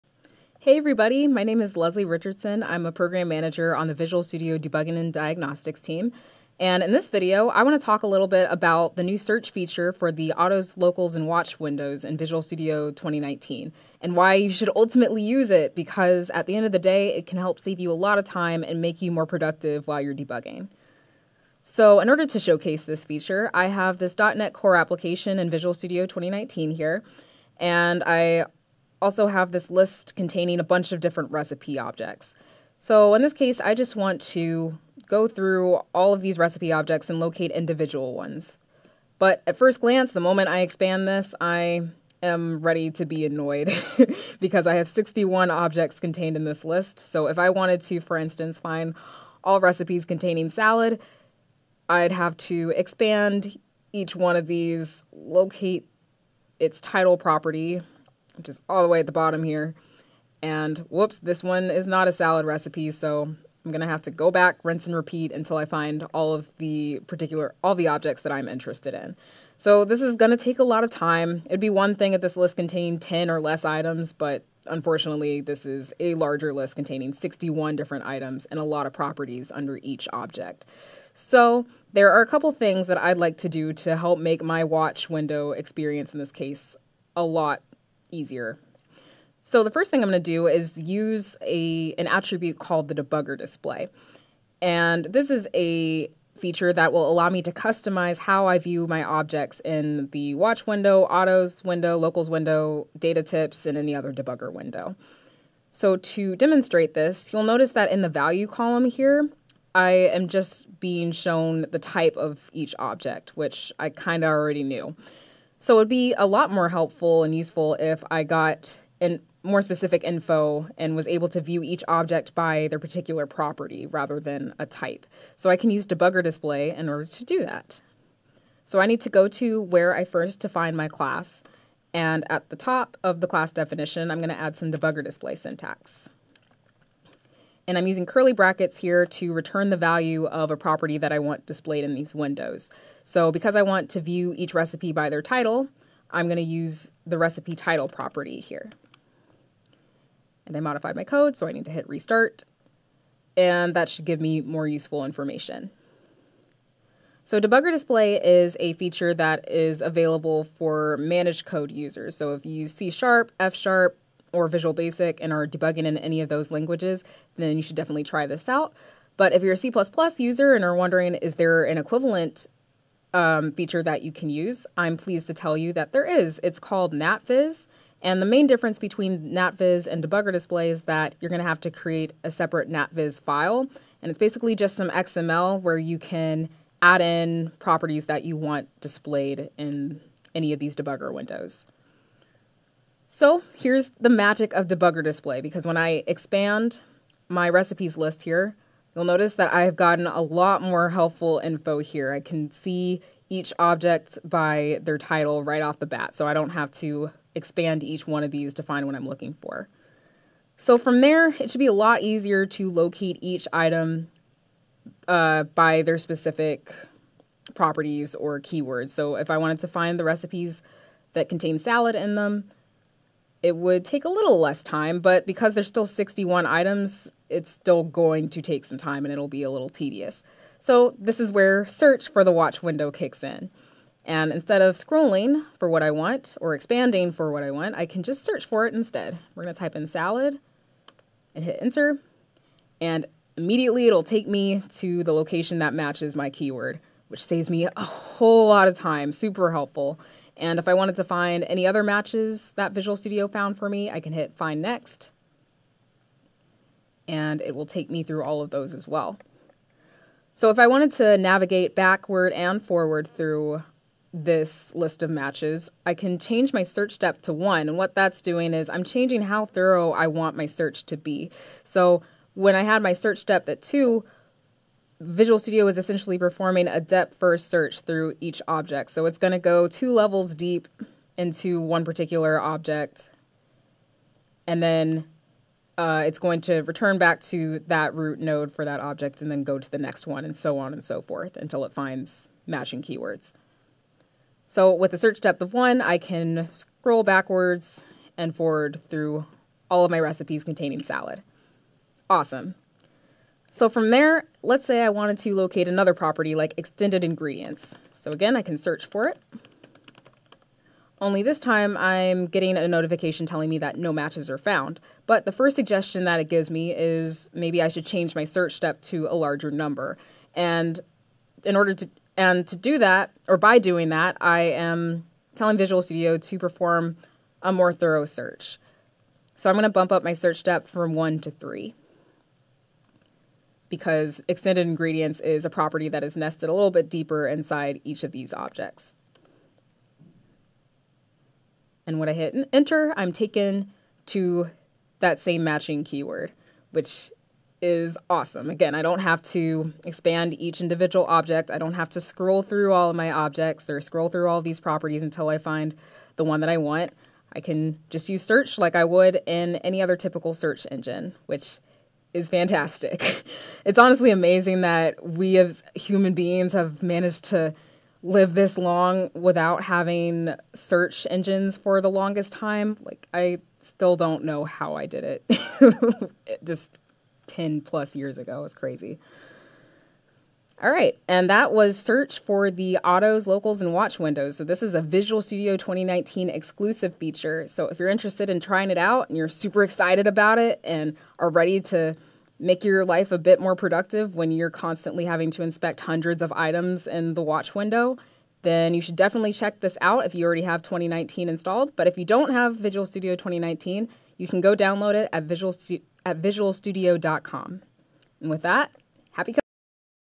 Save Time Debugging with the New Search Feature in Visual Studio
Leslie shows how you can save time debugging with the new Search feature for the autos, locals, and watch windows in Visual Studio 2019.